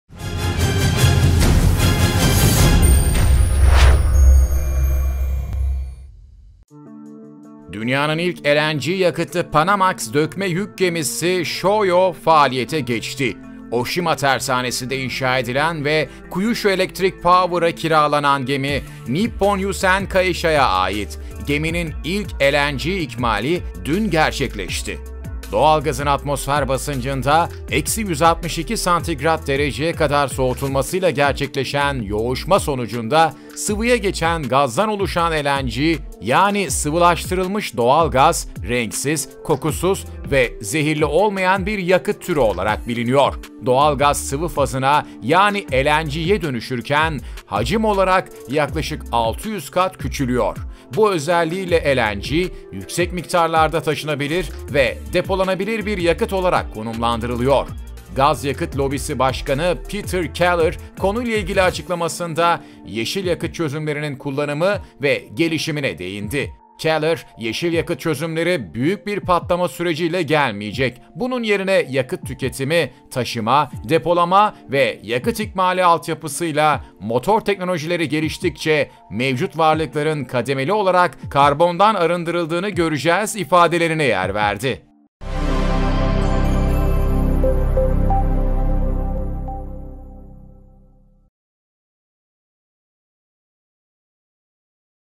Dünyanın İlk LNG Yakıtlı Panamax'ı Faaliyete Geçti
Dünyanın ilk LNG yakıtlı Panamax dökme yük gemisi Shoyo faaliyete geçti. Oshima Tersanesi'nde inşa edilen ve Kyushu Electric Power'a kiralanan gemi, Nippon Yusen Kaisha'ya ait. Geminin ilk LNG ikmali dün gerçekleşti.